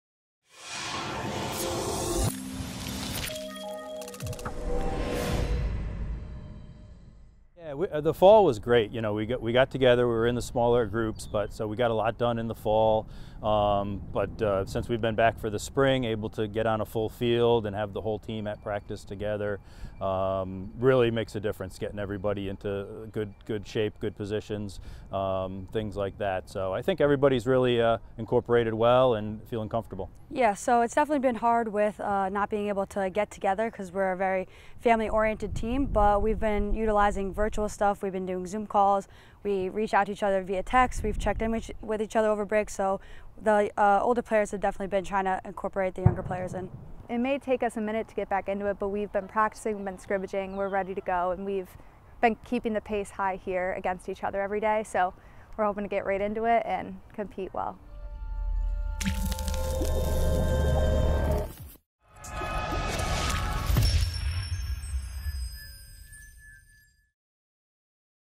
WSOC: Spring 2021 Season Preview
Head coach Steve Karbowski and captains Deanna Lomino and Sophia Dimkopoulos preview the Spring 2021 Women's Soccer season